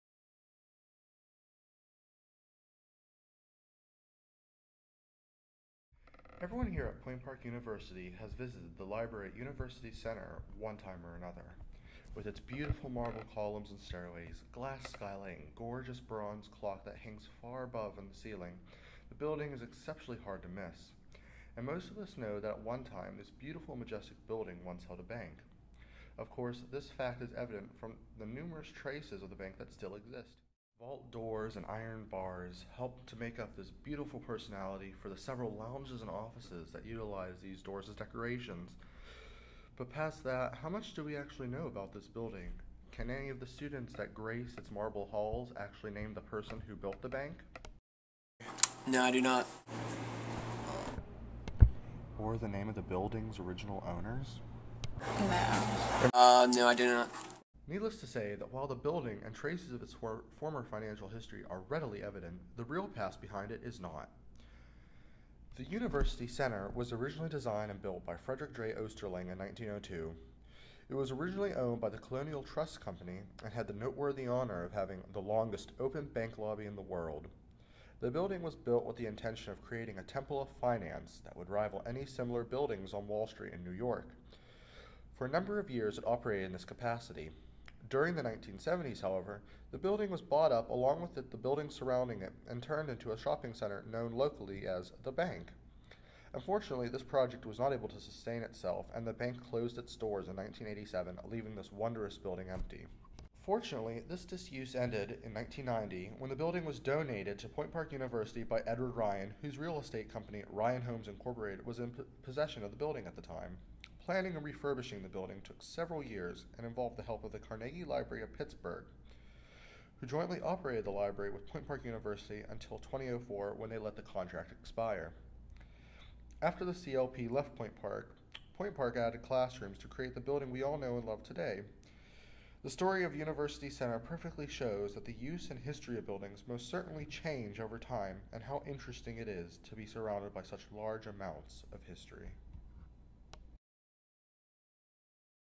A Brief History of University Center at Point Park University
A video project for my multimedia class about the history of the campus library.